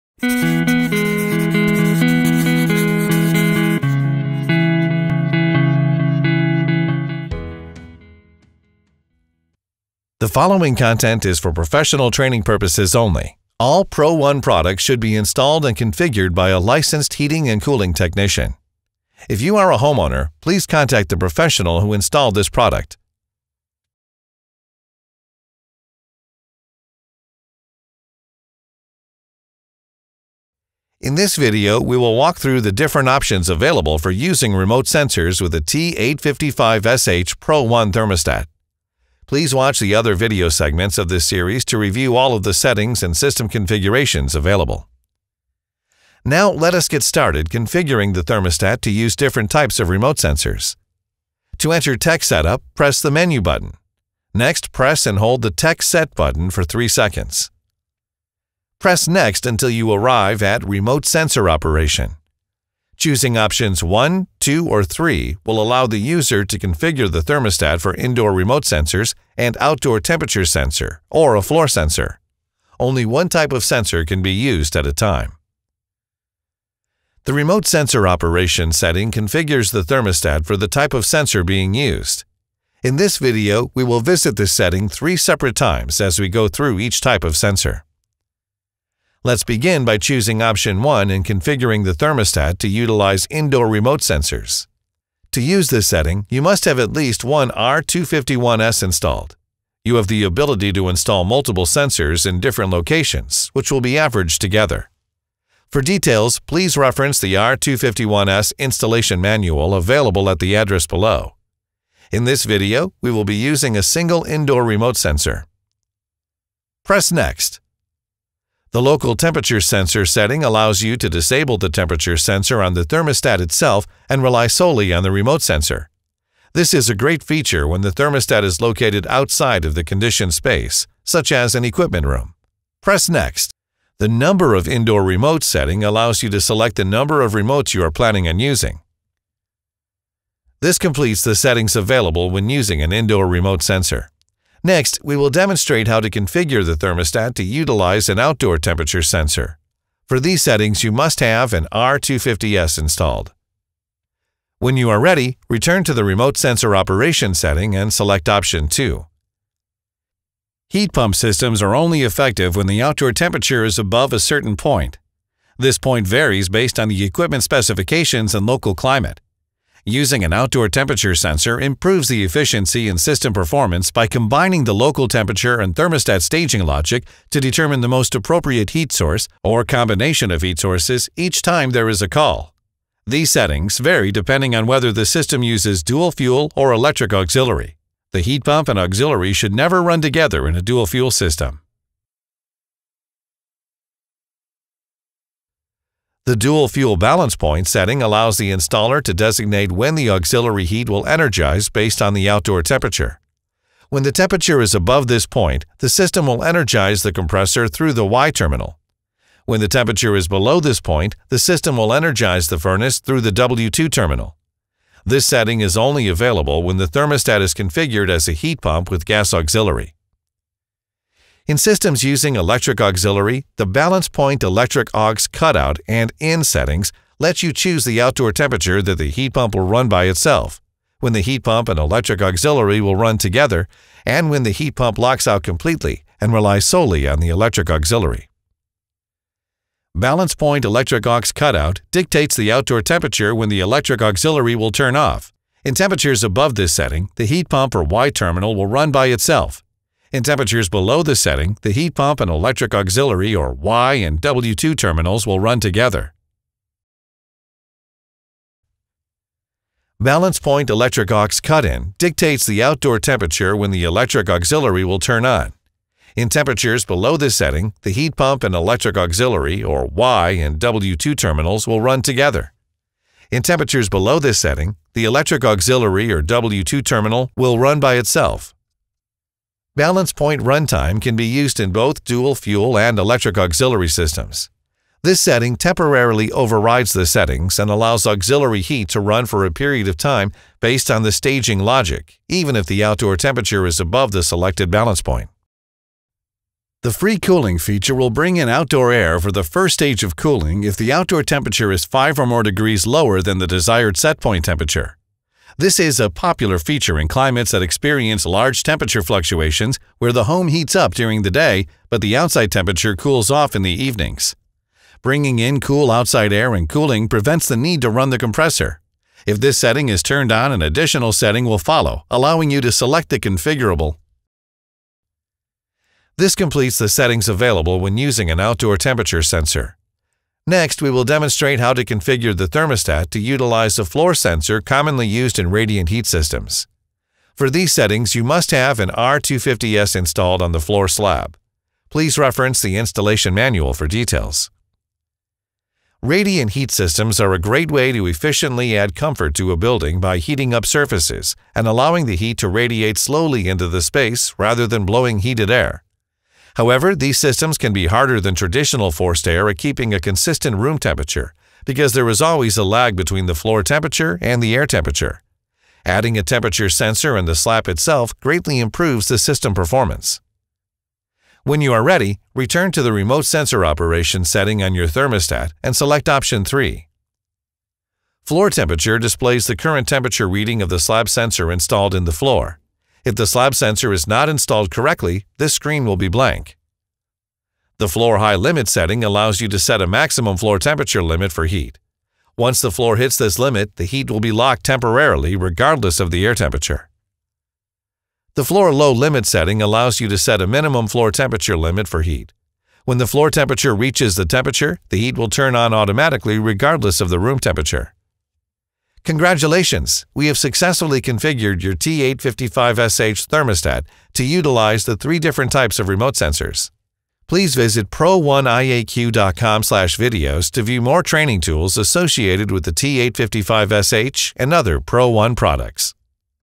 How To Set Up A PRO1 Thermostat - Using Remote Sensors (T855SH)
In this video we will outline how to configure a PRO1 T855SH thermostat for remote sensors.